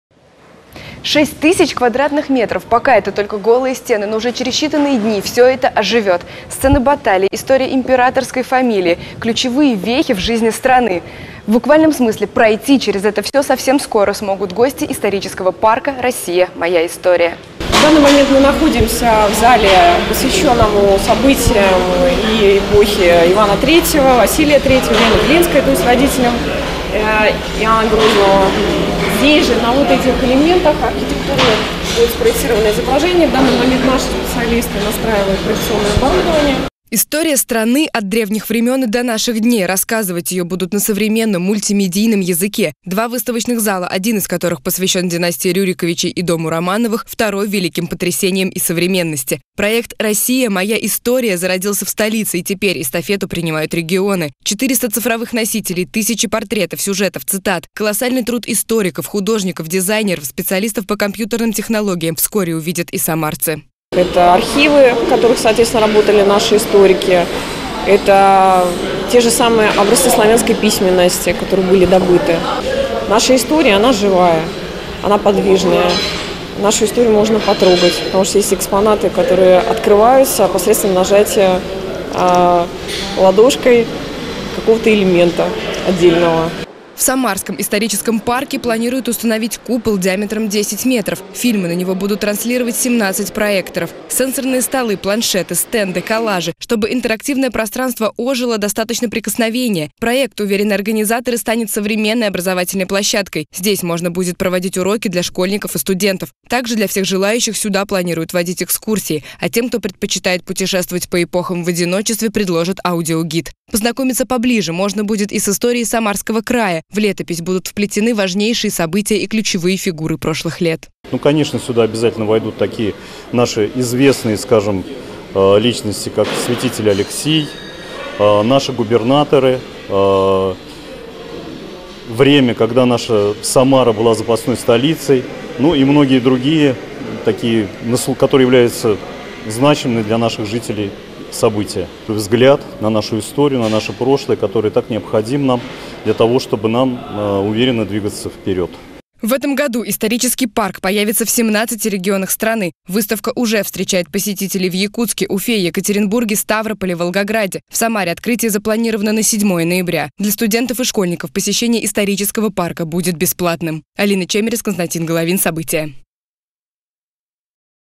В Самаре откроется "Исторический парк"
Хроника прошлого в формате мультимедиа. В Самаре откроется Исторический парк. Интерактивные стенды, сенсорные столы, огромные экраны - узнать, что происходило в России много веков назад, можно будет с помощью одного прикосновения. Проект уже нашел миллионы поклонников в Москве, теперь добрался и до регионов. Самарский исторический парк сейчас на стадии монтажа.